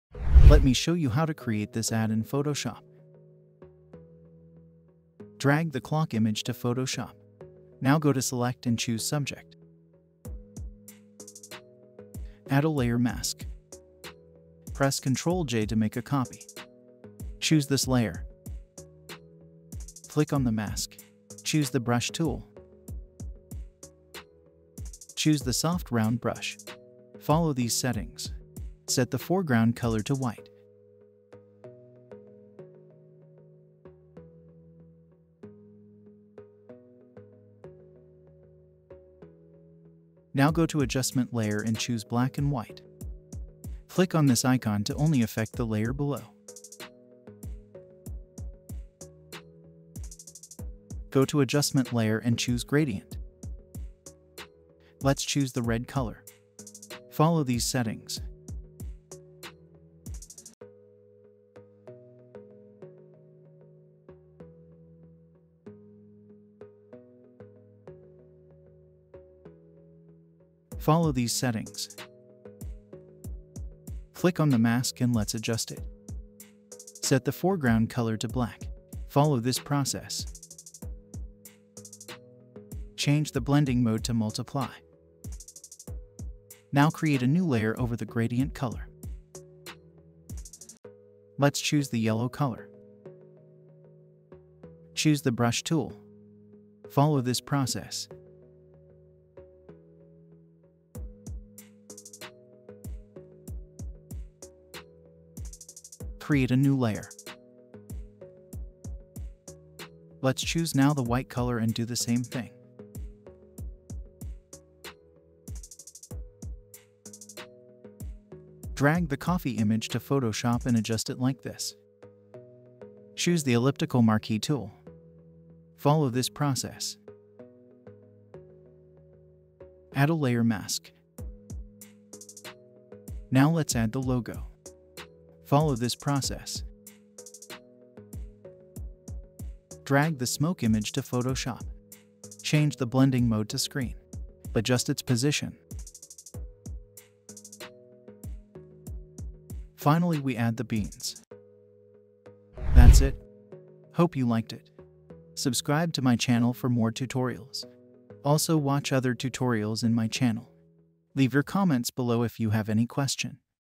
How to Design Ads in Photoshop
In this tutorial, i will show you how to design an ad in adobe photoshop.